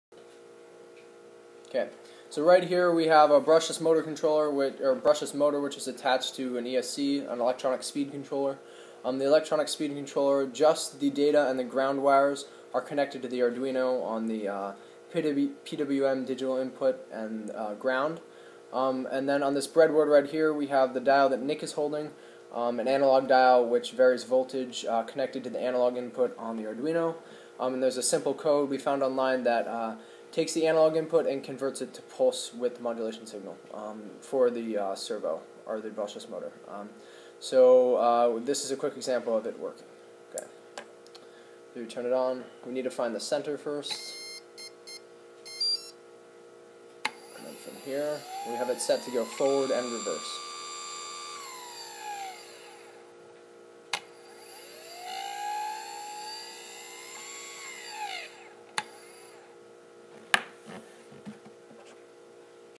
Robo Goby -- ESC with Arduino Uno
This is an example of an Arduino Uno controlling a brushless motor using an Electronic Speed Controller (ESC).